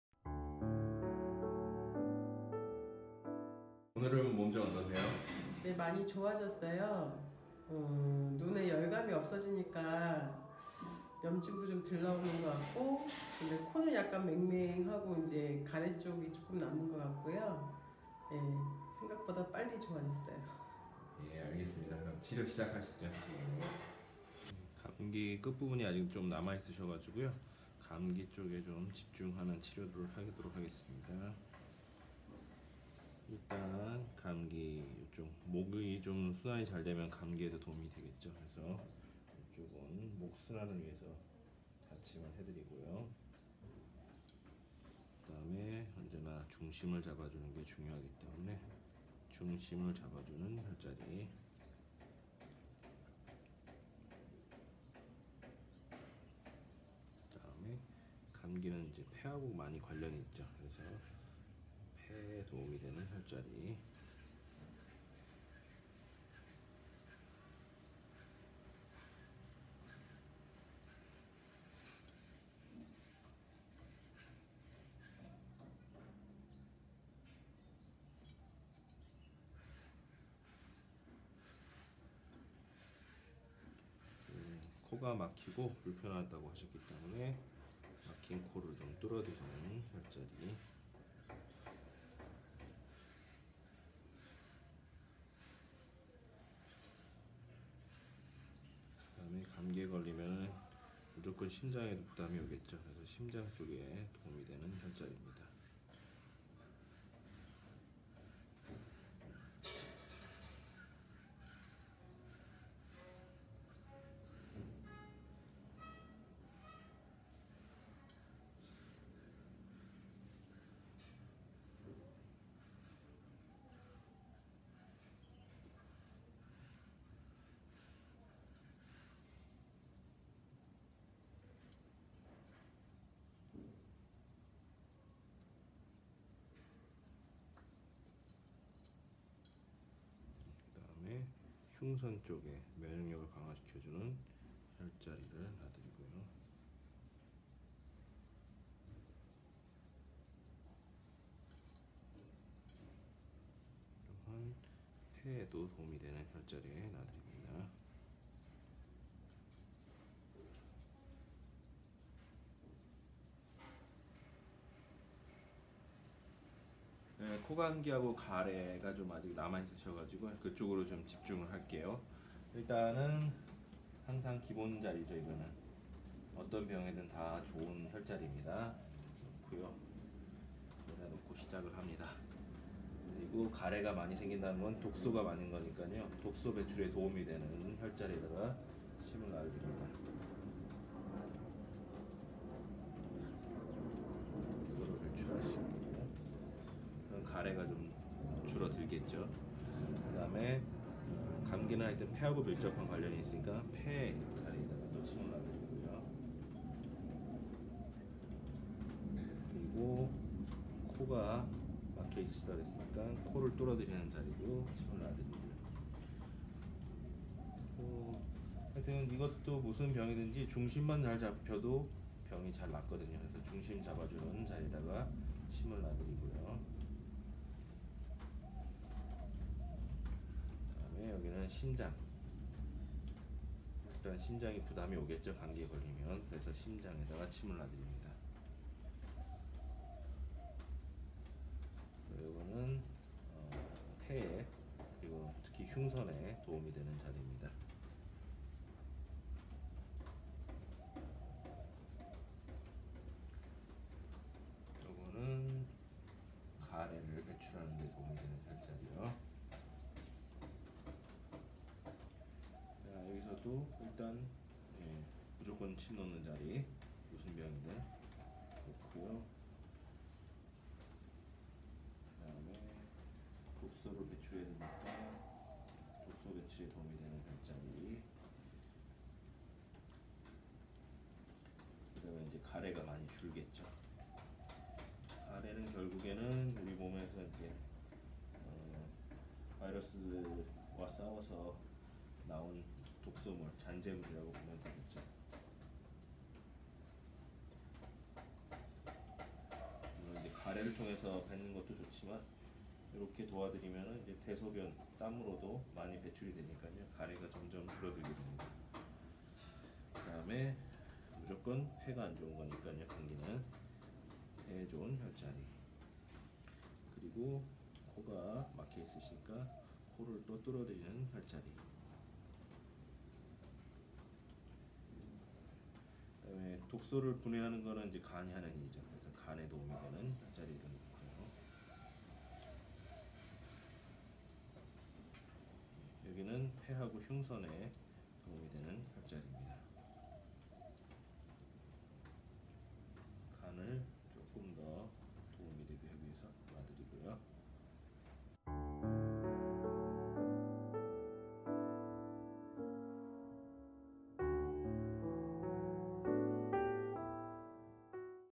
임상기록 - 눈물, 허리통증, 비염 13편(가래) - 힐링침 / 균형요법 / 내손은약손 - Acupuncture Points
더 자세한 정보는 제 블로그에서 얻을 수 있습니다.